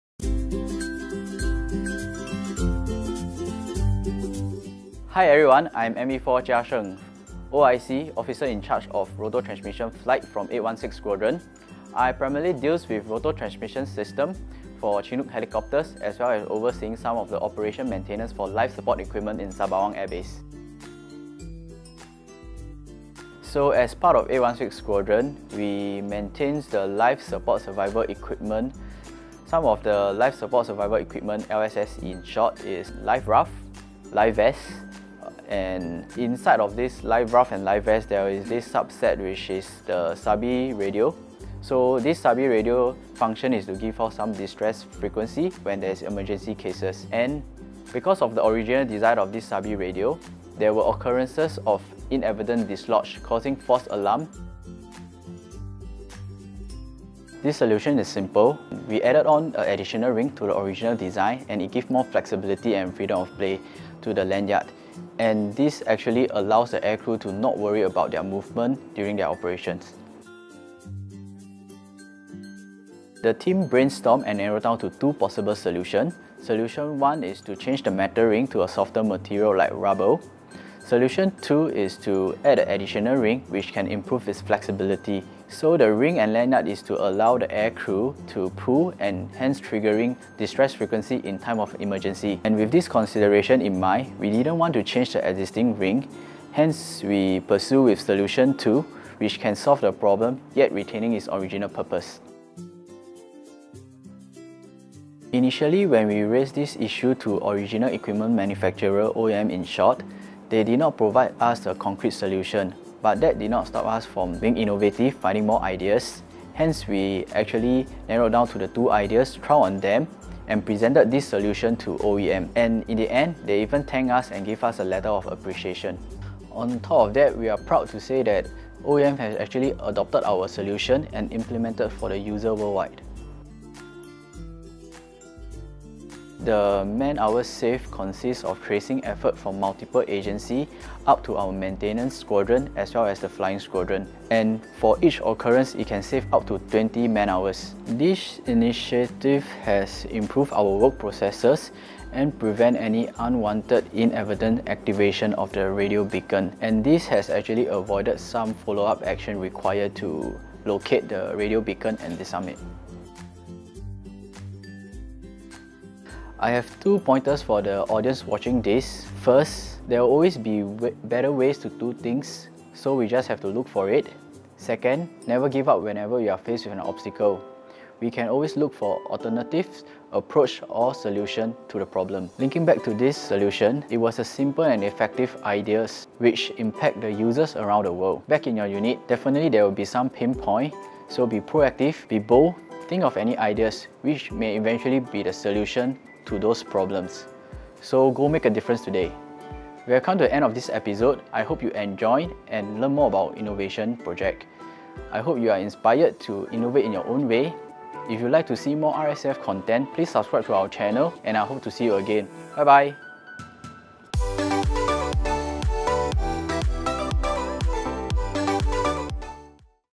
Enhanced Reliability of Aircrew Beacon (An RSAF Innovation)
Innovation is at the heart of the RSAF's dynamic culture! 816 SQN tells us about their latest bright idea which has since been ADOPTED WORLDWIDE by the Original Equipment Manufacturer (OEM) 😲 Find out what exactly it is in this video.

Video by REC Ong Chin Wee